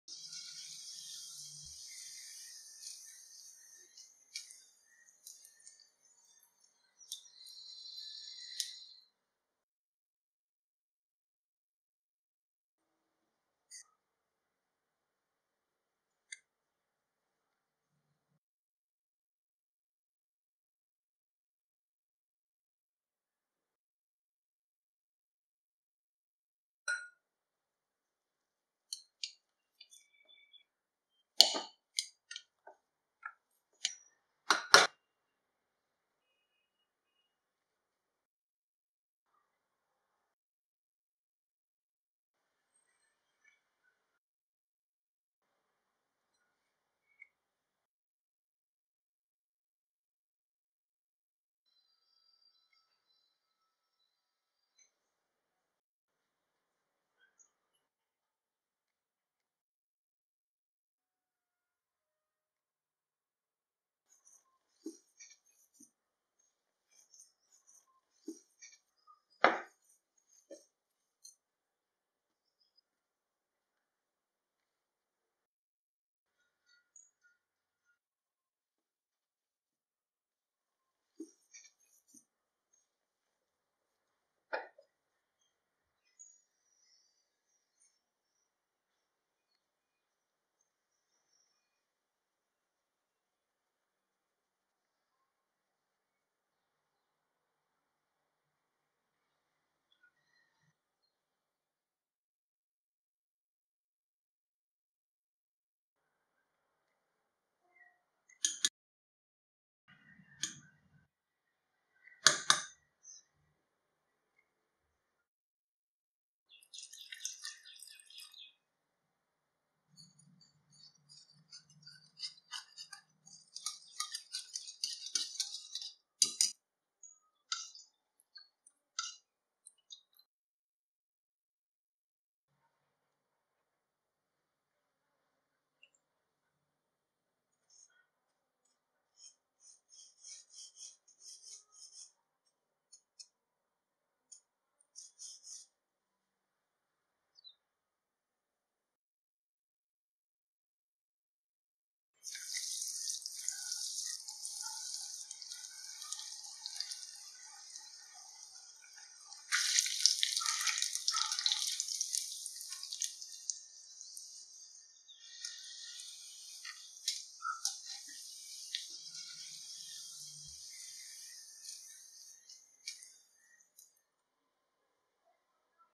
Crispy & Tasty Bread Roll Recipe/ Easy Iftar Snacks Recipe/ Bread Recipes
Bread Roll

Ingredients
Bread slices – 7
Boiled potato – 3 (small size)
Chat masala – ½ teaspoon
Onion – half
Grated carrot – 1
Coriander leaves
Grated ginger – 1 teaspoon
Green chilli – 1
Bread crumbs
Maida – 2 tbl spoon
Corn flour – 2 tbl spoon
Salt
Oil